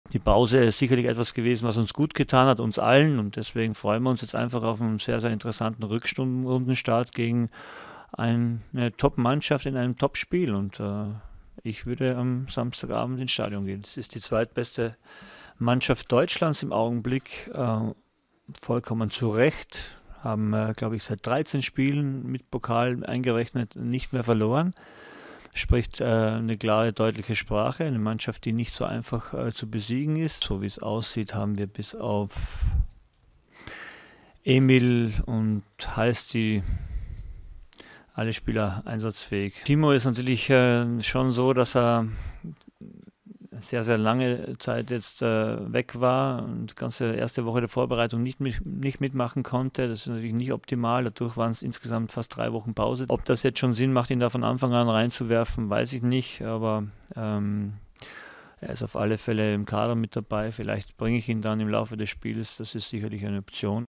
Werner-Comeback: "Er wird gegen Schalke im Kader sein"
Leipzigs Ralph Hasenhüttl muss beim Rückrundenstart am Samstag gegen Schalke 04 zu Beginn wahrscheinlich auf den zuletzt erkälteten Timo Werner verzichten. Definitiv nicht dabei sind Nationalspieler Marcel Halstenberg und Spielmacher Emil Forsberg.